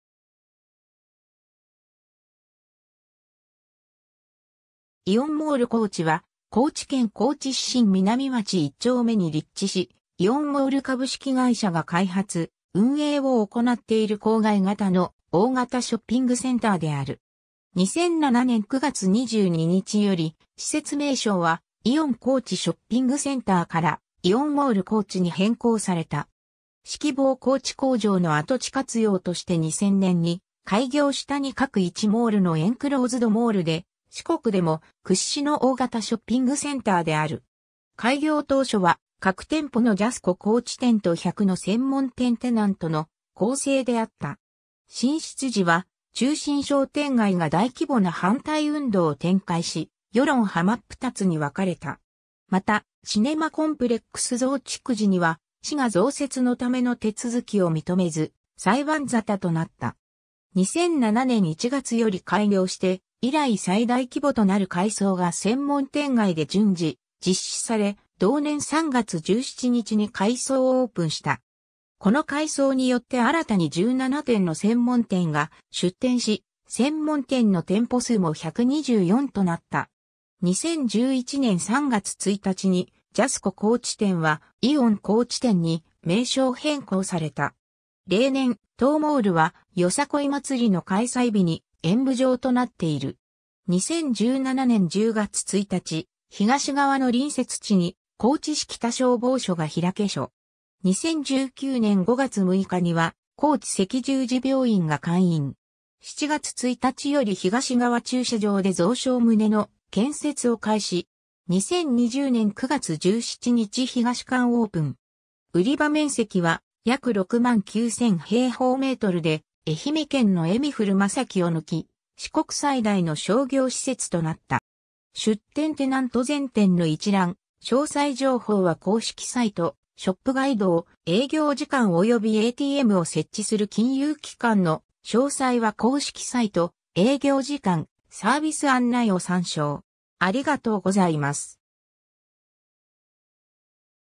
イオンモール高知
イオンモール高知（イオンモールこうち）は、高知県高知市秦南町一丁目に立地し、イオンモール株式会社が開発・運営を行っている郊外型の大型ショッピングセンターである。
 2007年（平成19年）9月22日より、施設名称は「イオン高知ショッピングセンター」から「イオンモール高知」に変更された。
 シキボウ高知工場の跡地活用として2000年（平成12年）に開業した2核1モールのエンクローズドモールで、四国でも屈指の大型ショッピングセンターである。
開業当初は核店舗のジャスコ高知店と100の専門店テナントの構成であった。
 進出時は中心商店街が大規模な反対運動を展開し、世論はまっぷたつに分かれた。
また、シネマコンプレックス（TOHOシネマズ）増築時には市が増設のための手続きを認めず、裁判沙汰となった。
 2007年1月より開業して以来最大規模となる改装が専門店街で順次実施され、同年3月17日に改装オープンした。
この改装によって新たに17店の専門店が出店し、専門店の店舗数も124となった。
 2011年（平成23年）3月1日にジャスコ高知店はイオン高知店に名称変更された。
 例年、当モールはよさこい祭りの開催日に演舞場となっている。
 2017年（平成29年）10月1日、東側の隣接地に高知市北消防署が開署。
 2019年（令和元年）5月6日には高知赤十字病院が開院。
7月1日より東側駐車場で増床棟の建設を開始。
 2020年（令和2年）9月17日東館オープン。
売場面積は約69,000m2で、愛媛県のエミフルMASAKIを抜き、四国最大の商業施設となった。
 出店テナント全店の一覧・詳細情報は公式サイト「ショップガイド」を、営業時間およびATMを設置する金融機関の詳細は公式サイト「営業時間・サービス案内」を参照。